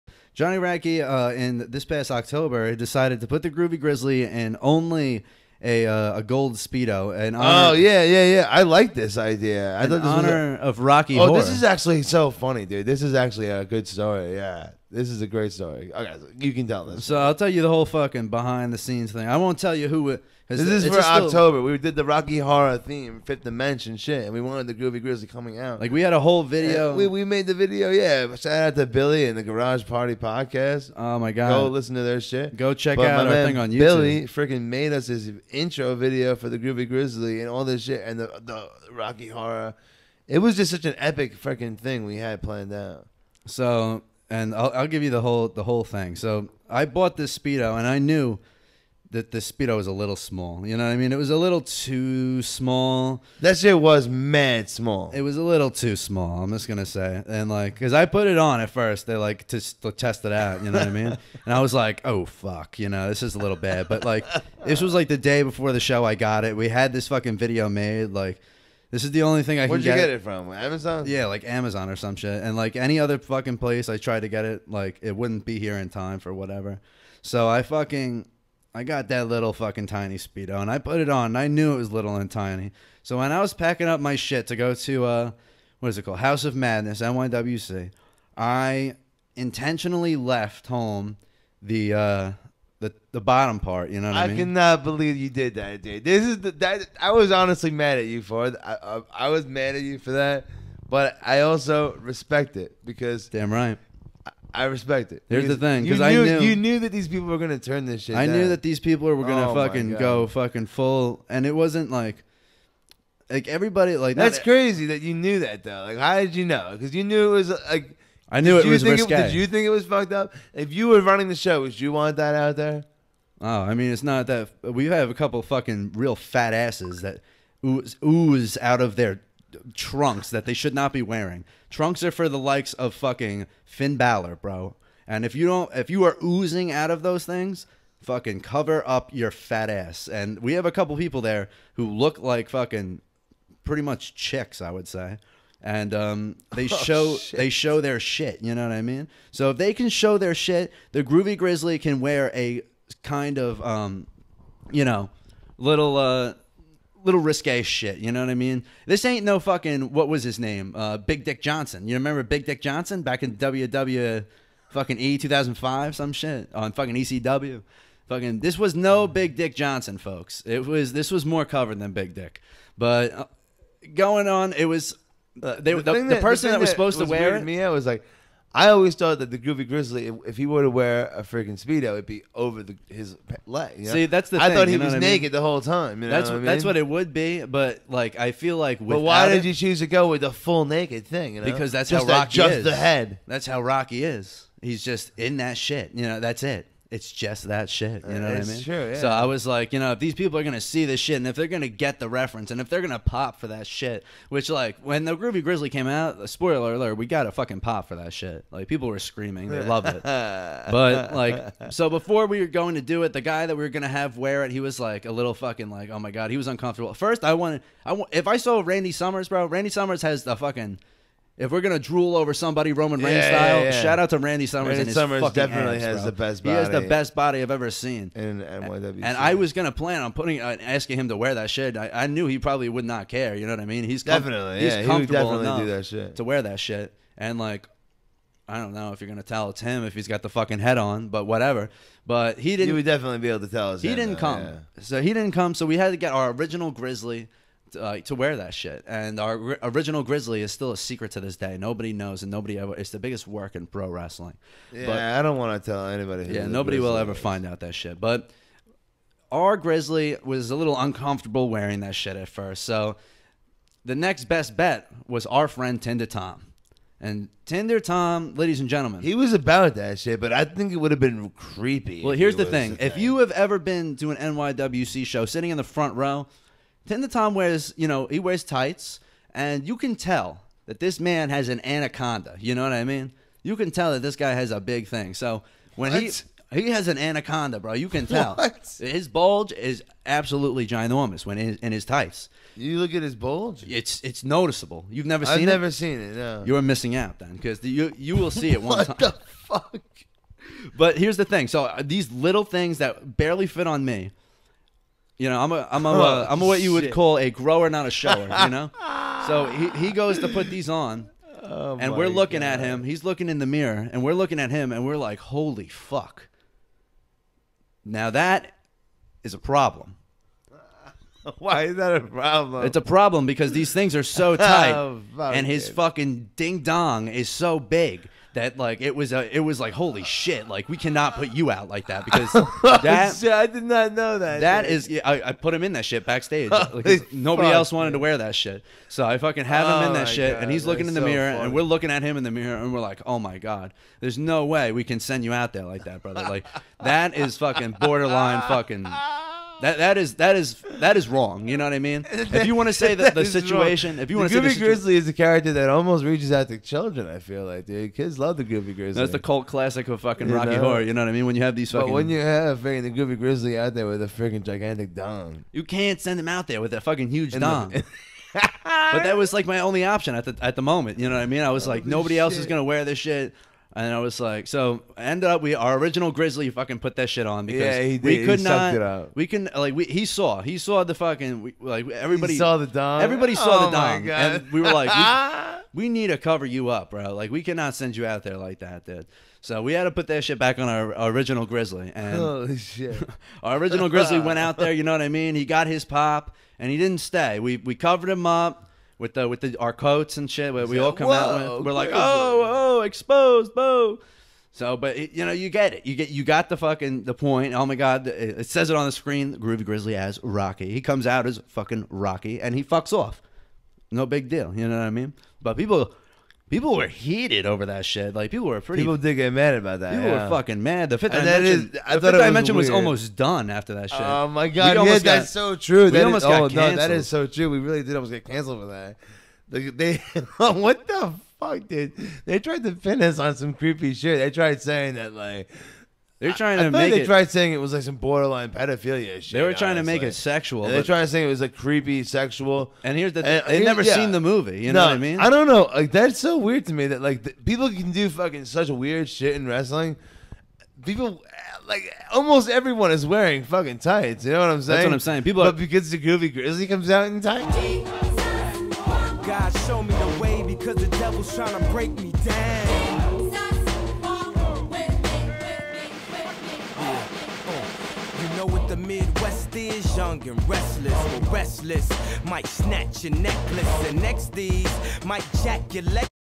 Rocky Horror (TSATB Highlights)
The 5th Dimension recreated RockyHorror as an entrance for their Halloween show. However, a little too much skin was shown that day.

SoundCloud.com/TheSquatchAndTheBun